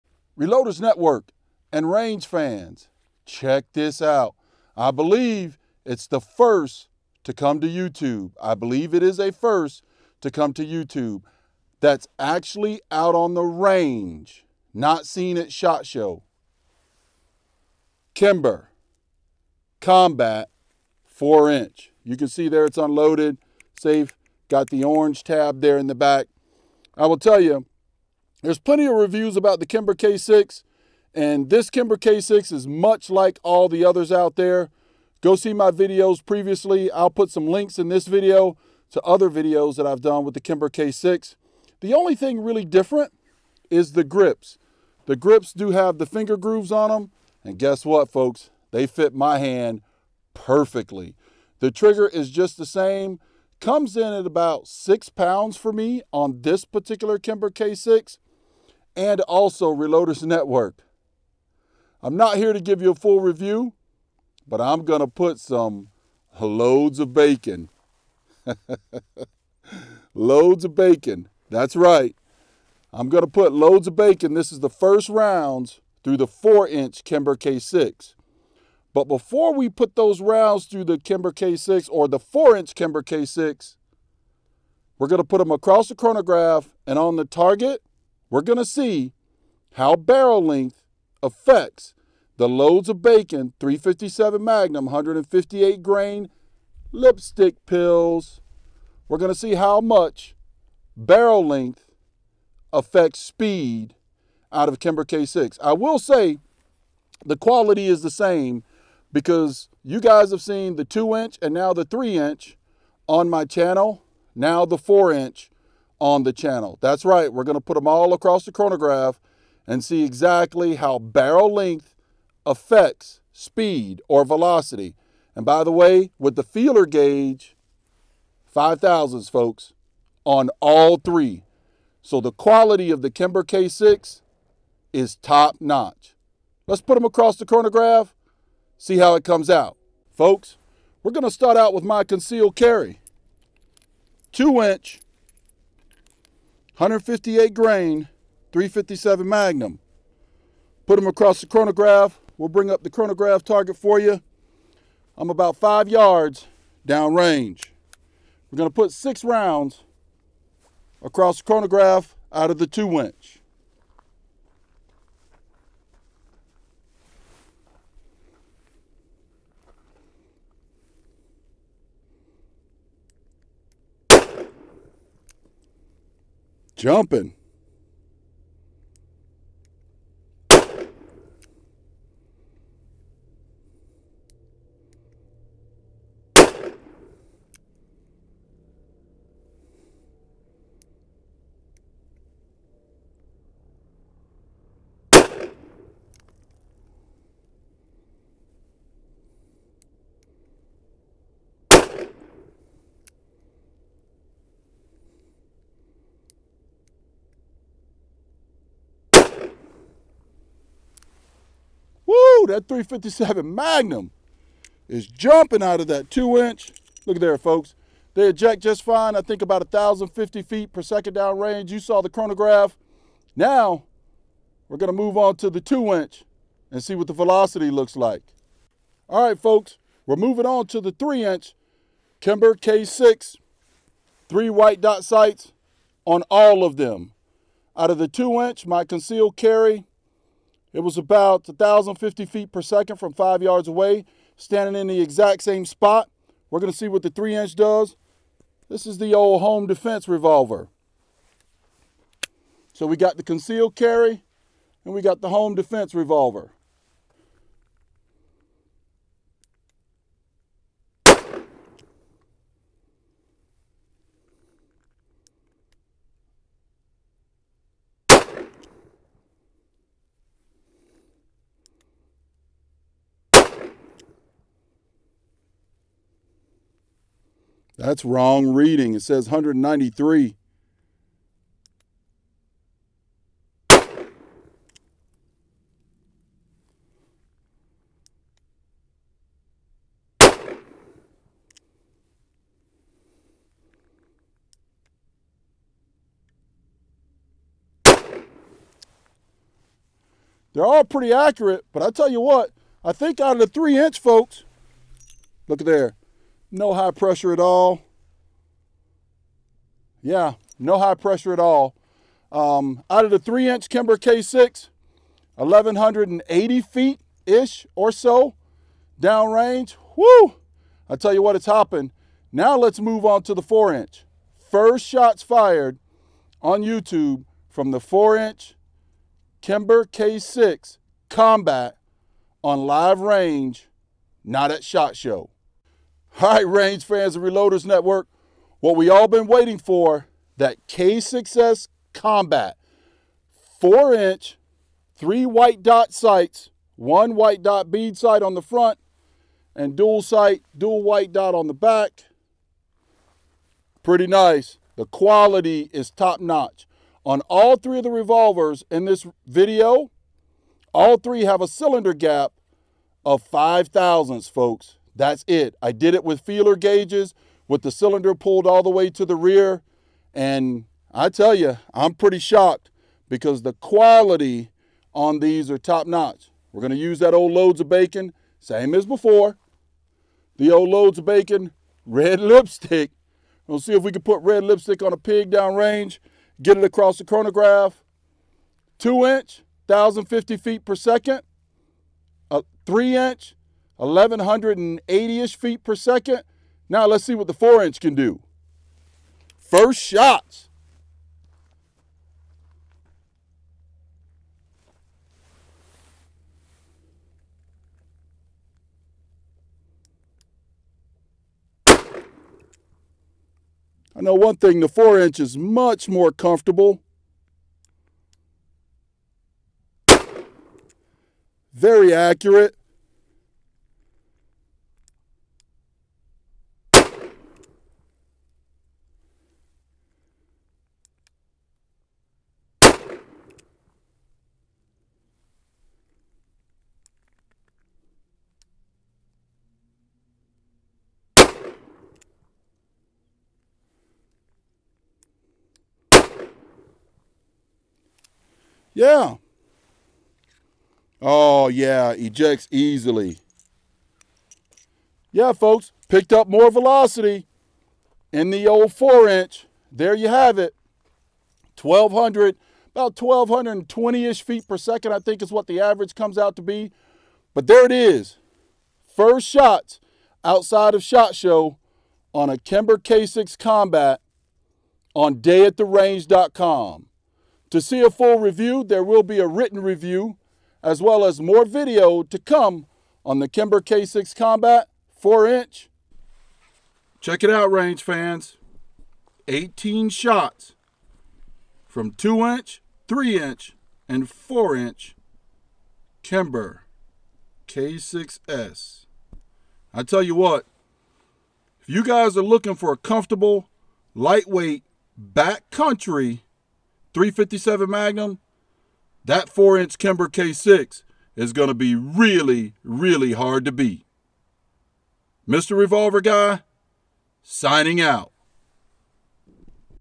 Kimber K6s Combat 4" DA/SA Revolver:Chronograph Results
With my search capability this is the first recorded shots fired out of the new 2020 Kimber K6s Combat 4inch outside of shot show.

I have posted a few reviews on this channel of the K6s and the quality is very consistent and very good on all the ones I have handled. What interested me the most was using the feeler gauge to measure barrel to cylinder gap and finding to my amazement that all of them were .005.

So what better way to test the effects of barrel length on velocity than with the same brand revolvers with equal quality.

Kimberk6s 2in Conceal Carry
Hi 1084fps
Low 1060fps
Avg 1074fps -- 6shots

Kimberk6s 3in Home Defense
Hi 1182fps
Low 1124fps
Avg 1157fps -- 5shots
Picked up 83fps over the 2in Kimber

Kimberk6s 4in Backwoods/Hiking Carry
Hi 1251fps
Low 1157fps
Avg 1207fps -- 6shots
Picked up 50fps over 3in Kimber